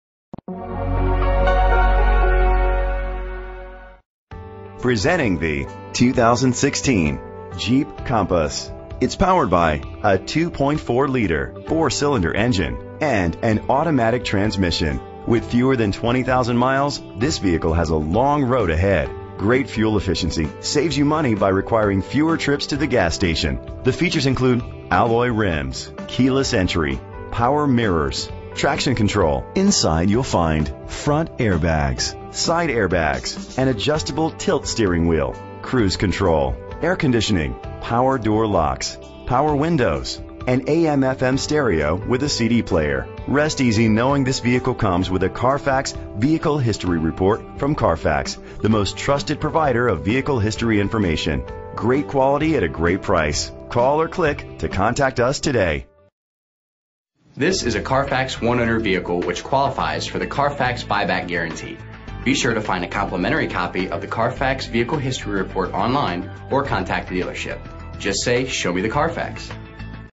2016 Jeep Compass F6917 - Tinley Park IL
Year: 2016
Make: Jeep
Model: Compass
Trim: Sport
Engine: 2.4 liter 4 Cylindercylinder
Transmission: Automatic
Color: Bright White Clearco
Mileage: 18181
Address:
17514 Oak Park Avenue
Tinley Park, IL 60477
VIN:1C4NJDBB5GD631659